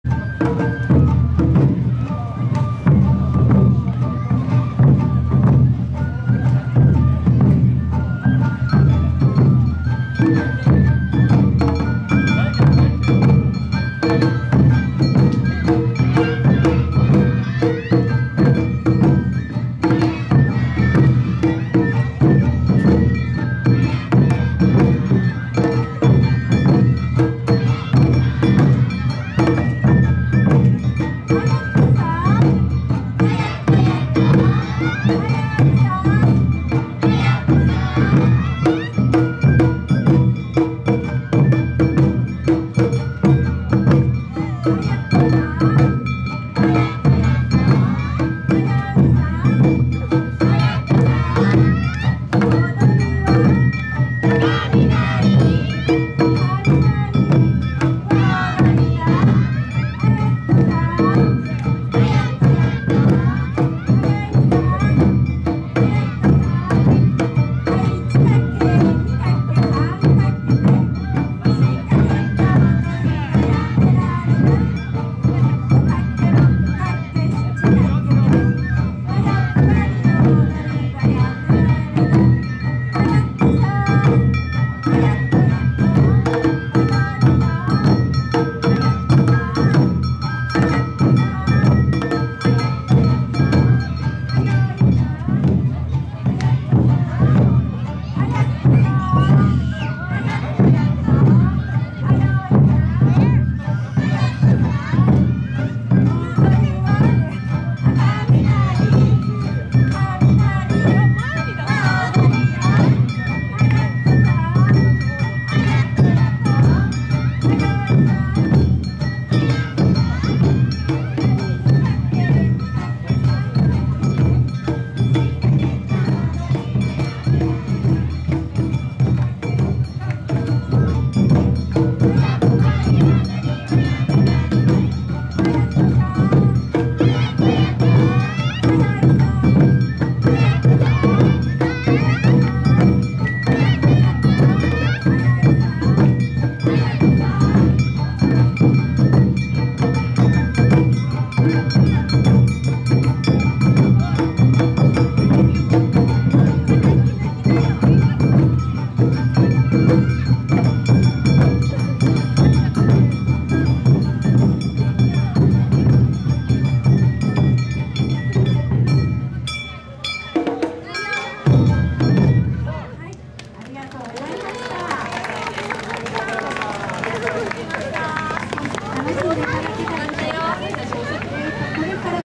浅草雷連 皆で阿波踊り教室／千束公園／いきいき浅草あじさい祭2019
2019/06/16（令和元年6月16日）
【祭り】いきいき浅草あじさい
【場所】千束公園、長國寺
【主催】一葉桜開運振興会（イベント実行委員会）
【後援】台東区
【協力】馬道地区町会連合会、清川地区町会連合会、奥浅草観光協会、浅草商店連合会、浅草観光連盟、東武鉄道、首都圏新都市鉄道、東京メトロ、東京都交通局、めぐりん
【協賛】浅草花やしき、ROXまつり湯